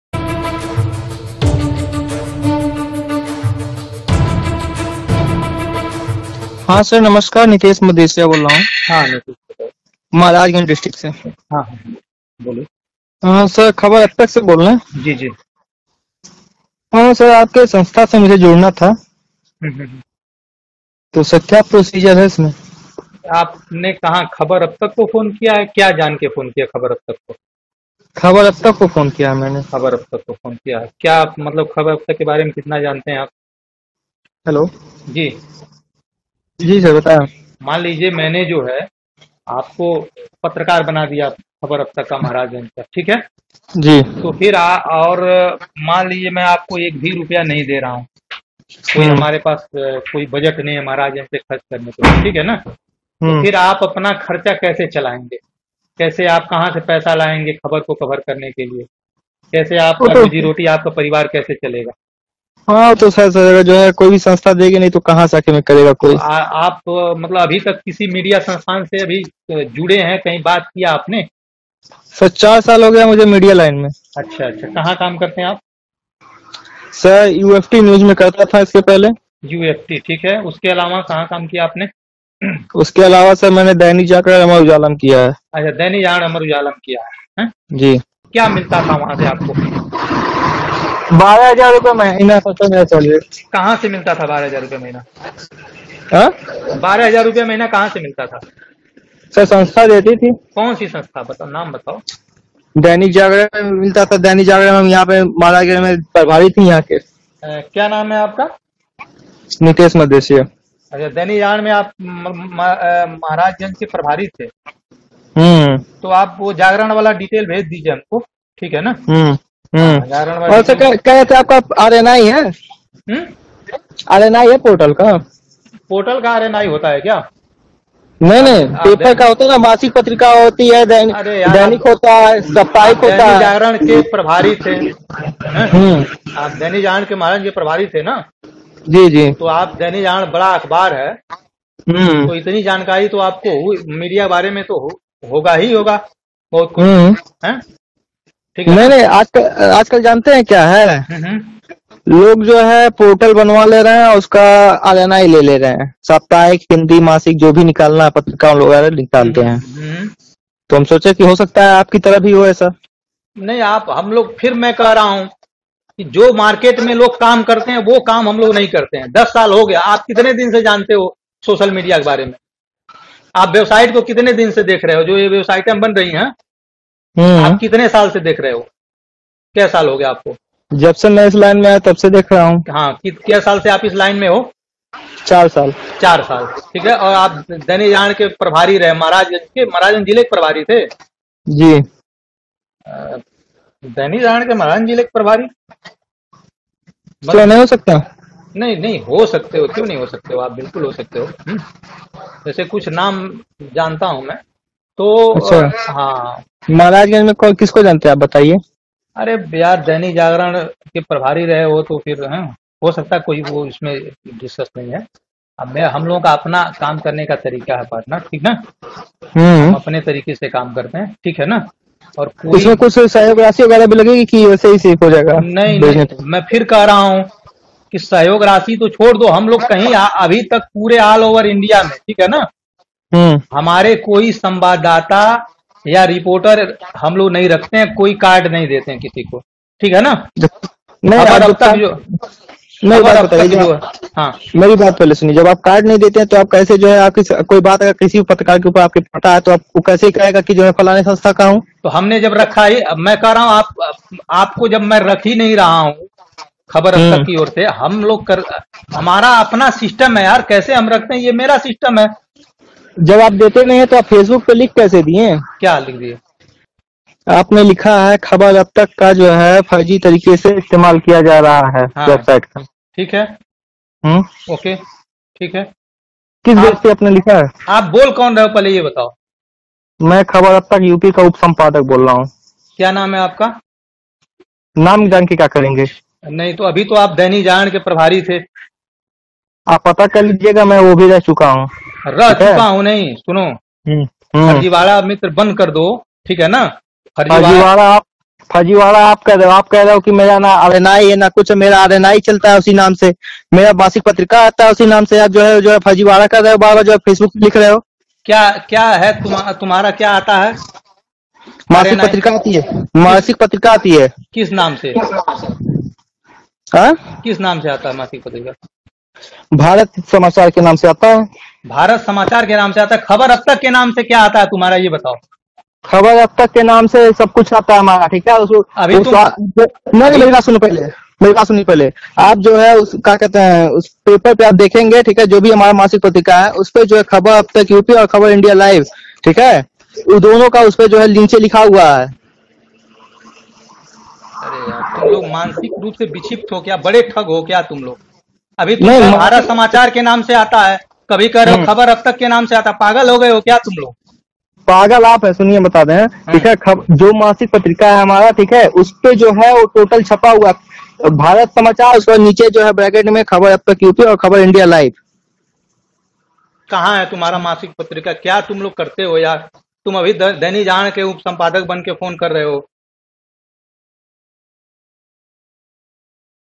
ऐसा जालसाज पत्रकार शायद ही पहले कभी आपने देखा हो, इसका पूरा सच जानकर आप हैरान हो जाएंगे
इस फर्जी पत्रकार का परिचय जानकर आपका सर चकरा जायेगा.. ये दैनिक जागरण अख़बार का प्रभारी रहा है. अमर उजाला में भी रहा है. भारत समाचार का मालिक है.. इसके साथ ही ख़बर अब तक का उप संपादक है.. और भी बड़े मीडिया संस्थानों का मालिक हो सकता है... पूरी बातचीत जरूर सुन लीजिए.